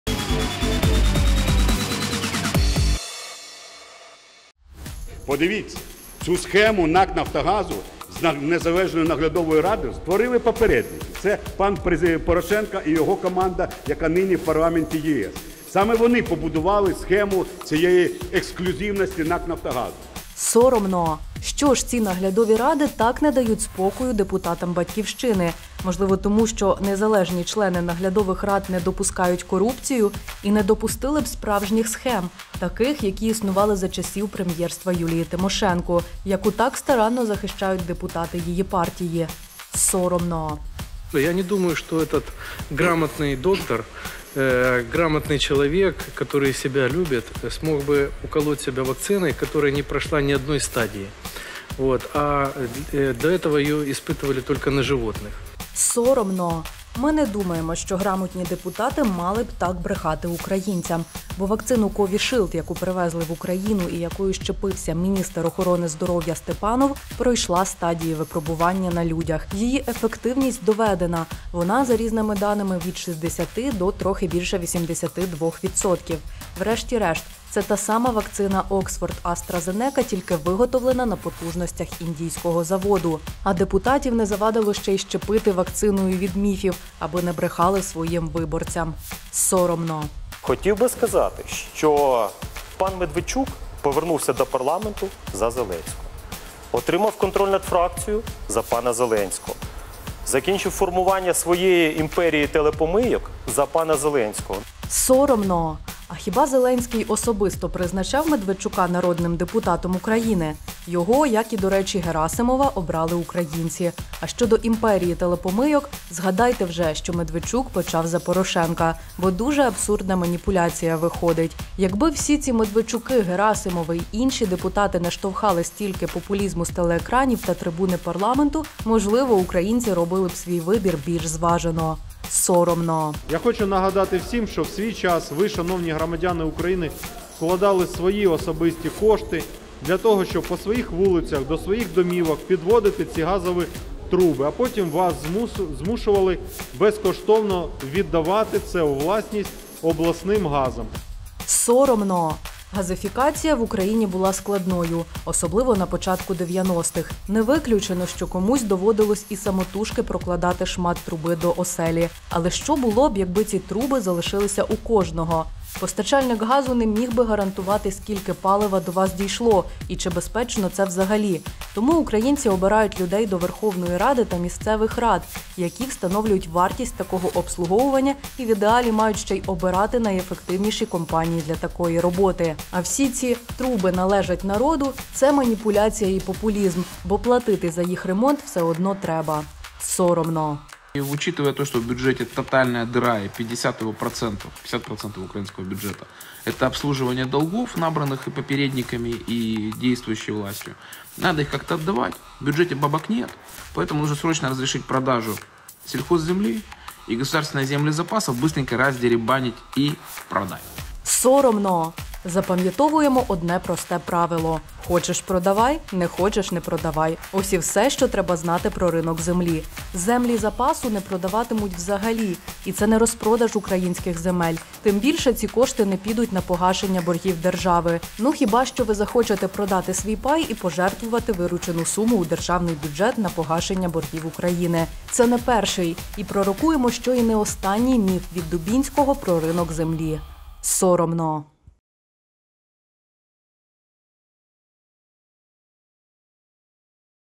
VoxCheck & Соромно. Качний, Кучеренко, Герасимов, Лубінець, Дубінський
00:00 – Олексій Кучеренко, депутат, фракція «Батьківщина»:
«Подивіться, цю схему НАК “Нафтогазу” з незалежною наглядовою радою створили попередники. Це пан прези... Порошенко і його команда, яка нині в парламенті “ЄС”. Саме вони побудували схему цієї ексклюзивності НАК “Нафтогазу”».

00:41 — Олександр Качний, депутат, фракція «Опозиційна платформа – За життя»:
«Я не думаю, що цей грамотний лікар, грамотна людина, яка себе любить змогла б вколоти собі вакцину, яка не пройшла жодної стадії. Ось, а до цього її випробовували тільки на тваринах».

01:30 — Артур Герасимов, депутат, фракція «Європейська Солідарність»:
«Хотів би сказати, що пан Медведчук повернувся до парламенту за Зеленського. Отримав контроль над фракцією за пана Зеленського. Закінчив формування своєї імперії телепомийок за пана Зеленського».

02:13 — Дмитро Лубінець, депутат, депутатська група «За майбутнє»:
«Я хочу нагадати всім, що у свій час ви, шановні громадяни України, вкладали свої особисті кошти для того, щоб по своїх вулицях до своїх домівок підводити ці газові труби, а потім вас змушували безкоштовно віддавати це у власність обласним газам».

03:10 — Олександр Дубінський, депутат, позафракційний:
«Враховуючи те, що в бюджеті тотальна діра і 50 його відсотків, 50% українського бюджету – це обслуговування боргів набраних і попередниками, і чинною владою, треба їх якось віддавати, а в бюджеті грошей немає, тому потрібно терміново дозволити продаж сільгоспземлі і державні землі запасу швиденько роздеребанити і продати».